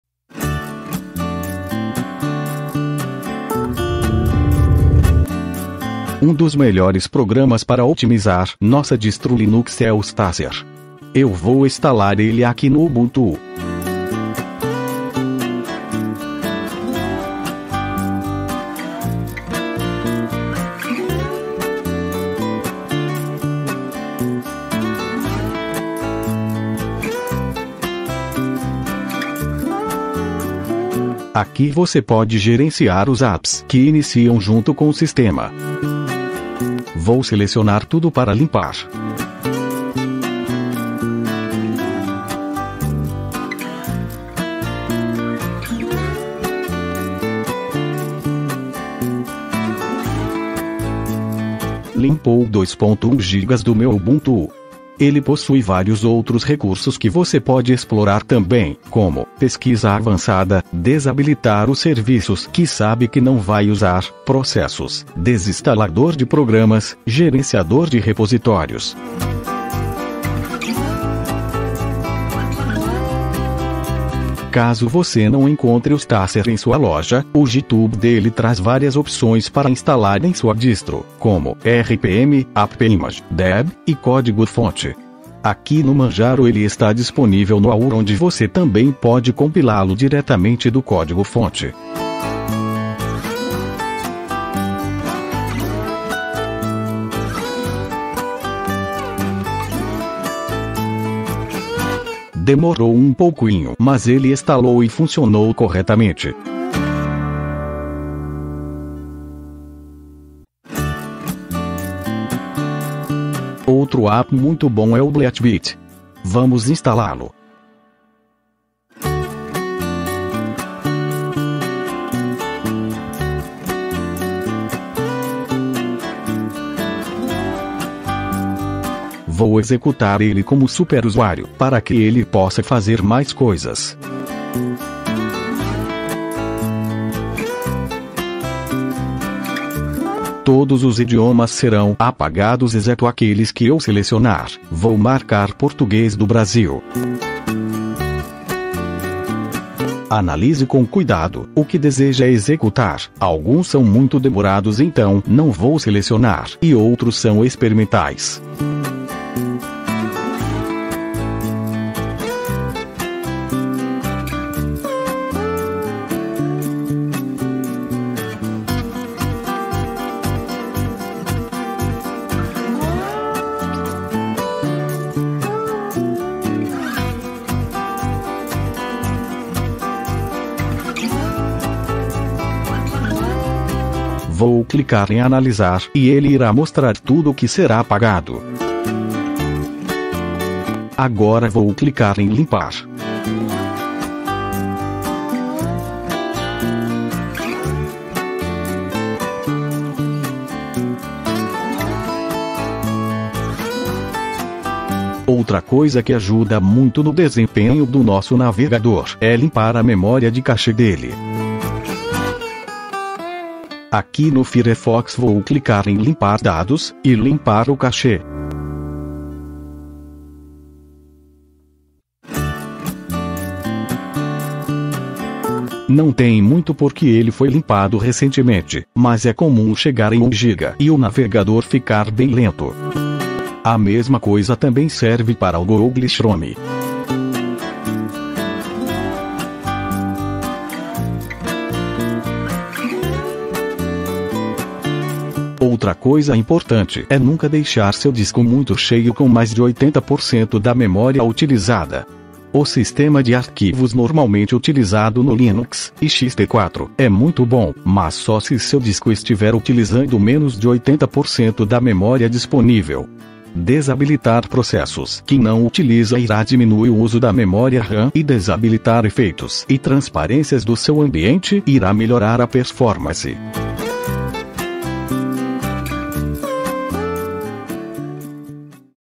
Como Otimizar Distros Linux
Não é tão comum a gente precisar otimizar as distribuições Linux porque dificilmente elas ficam lentas com o tempo, mas neste vídeo trago alguns programas e dicas de como otimizar sua Distro Linux.